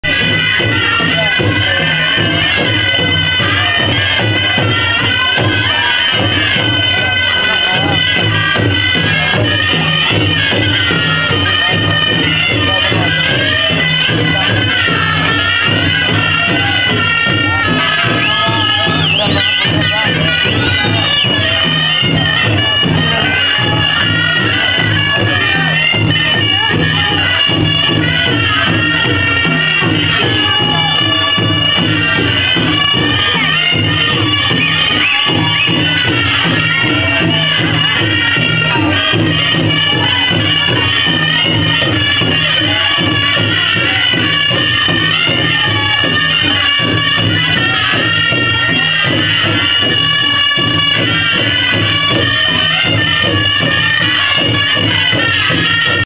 Les Hmadcha d'Essaouira-2
Les Hmadcha d'Essaouira - Moussem mai 2011
filmé avec appareil photo
Je voulais mettre un lien qui explique un peu ce qu'ils sont, mais rien sur wiki, donc rapide présentation via un autre site :